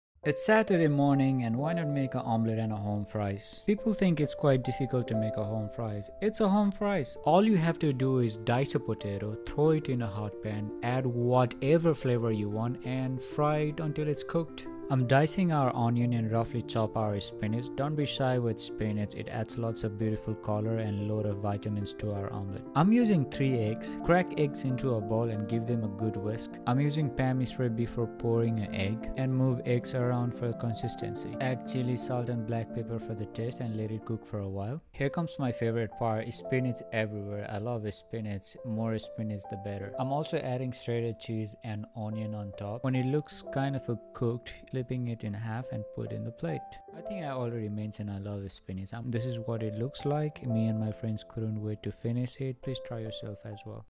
Spinach Onion Cheese Omelette: A Savory Breakfast Delight!
Join me in the kitchen as we whip up a mouthwatering spinach, onion, and cheese omelet that's bursting with flavor! We'll start by sautéing fresh spinach and onions until they're tender and fragrant, then fold them into fluffy eggs with a generous sprinkle of gooey cheese.

But that's not all - we're also serving up crispy home fries seasoned to perfection, providing the perfect savory accompaniment to our hearty omelet. For a refreshing twist, we're tossing together a simple yet satisfying side dish of salted spinach, adding a touch of brightness to our brunch spread.

Whether you're cooking up a hearty breakfast for yourself or entertaining guests, this spinach, onion, and cheese omelet with home fries and salted spinach is sure to impress. So grab your skillet and let's get cooking!